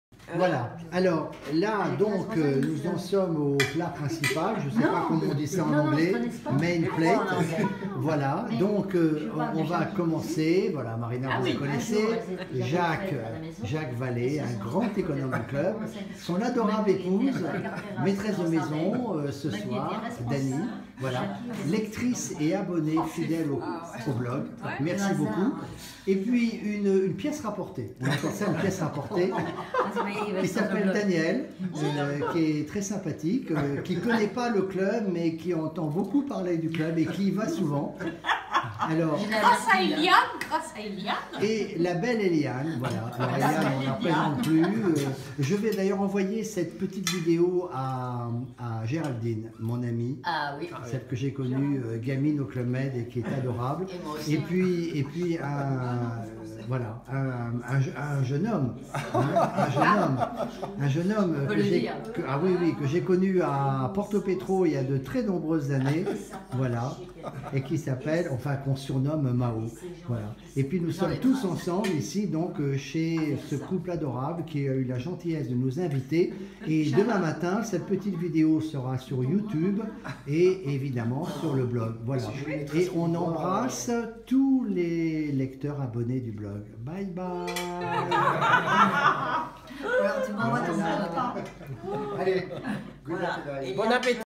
Dîner chez Dany et Jacques W.
Dîner très sympathique chez Dany et Jacques W. (le 18/11/2015)